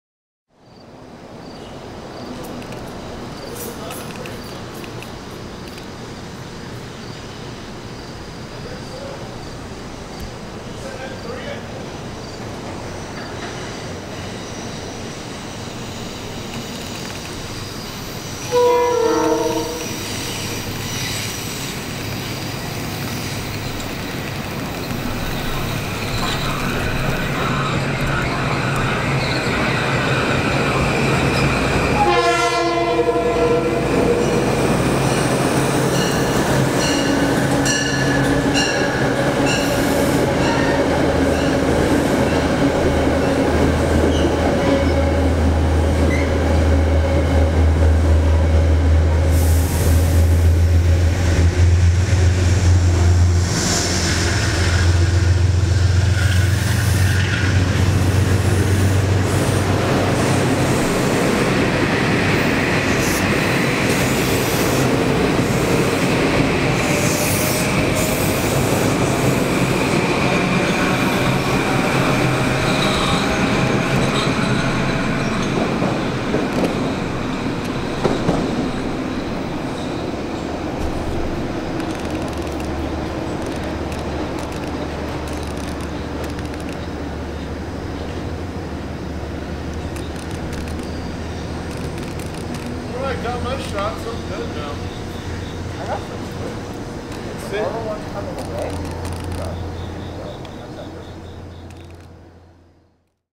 MTA Metro-North Railroad M8 PTC Test Train at Marble Hill - August 27, 2020
Here is a video taken on August 27, 2020, featuring a four-car train of MTA Metro-North Railroad Kawasaki Heavy Industries M8 cars at Marble Hill on the Hudson Line. M8 trains do not generally run on the Hudson Line or the Harlem Line, but to test recently installed positive train control (PTC) systems, an M8 train with PTC onboard systems was sent up the Hudson Line.